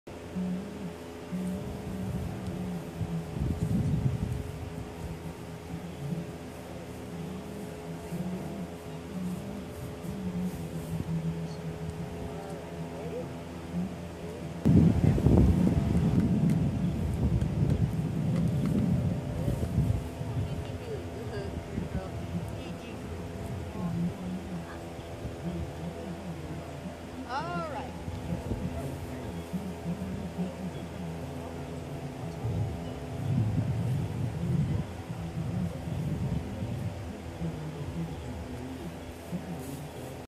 Skooter Taking a Bow - APHA Shesa Hot Lil OooLaLa
Skooter is being offered for sale by PaintedRockRanchTX.com

You may see more photos and info about Skooter by going to her webpage